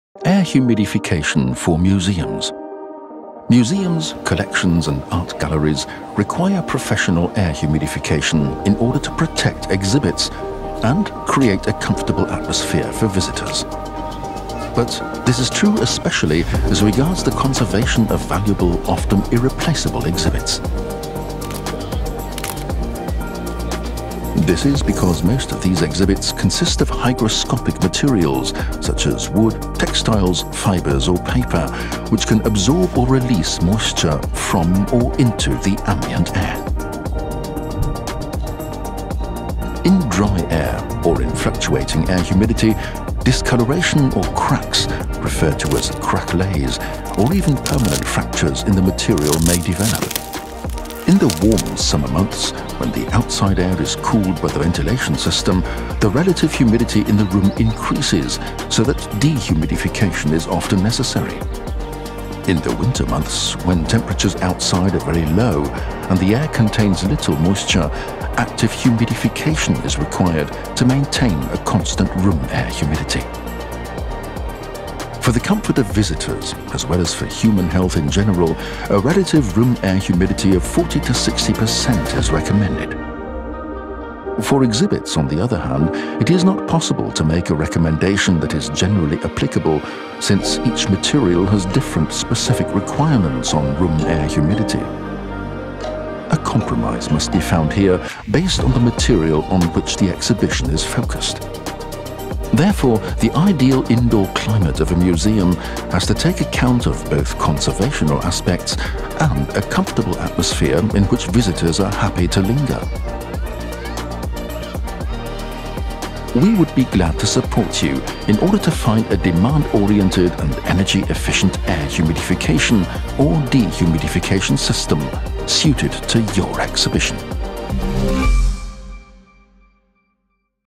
Humidification for museums and art galleries
Museums are charged with the task of preserving treasures of humanity and art. The innumerable, often unique and irreplaceable exhibits are worth many billions of euros and must therefore be permanently protected from any possible damage, especially from environmental influences. In this respect, the ability to control the often heavy loads imposed by museum visitors, who release heat, moisture, particles and pollutants into the indoor air, plays a key role.

In order to meet and overcome these challenges, most museums nowadays operate complex air conditioning systems — often several ones at once for different requirements in different areas. Only by operating these facilities is it possible to meet the conservation requirements for the protection of works of art in the long term and to ensure a comfortable, pleasant environment and good air quality for visitors.

Condair GmbH
Parkring 3
85748 Garching